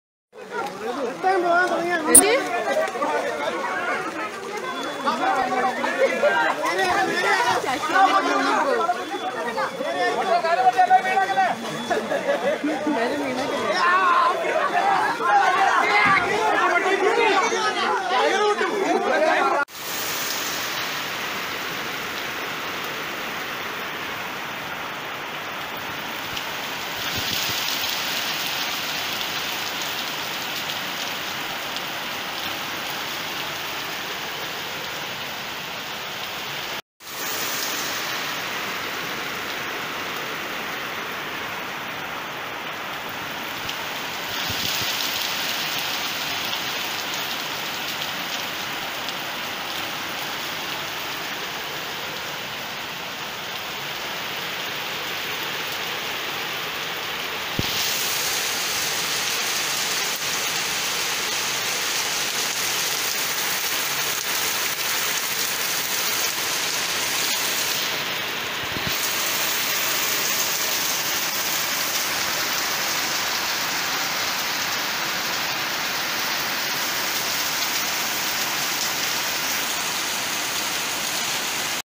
KOTTAYAM RAIN / MUNDAKAYAM RAIN / KSRTC UNDER WATER
കോട്ടയം മുണ്ടക്കയത്തു അതിശക്തമായ മഴ , പൂഞ്ഞാറിൽ KSRTC വെള്ളത്തിൽ അകപ്പെട്ടു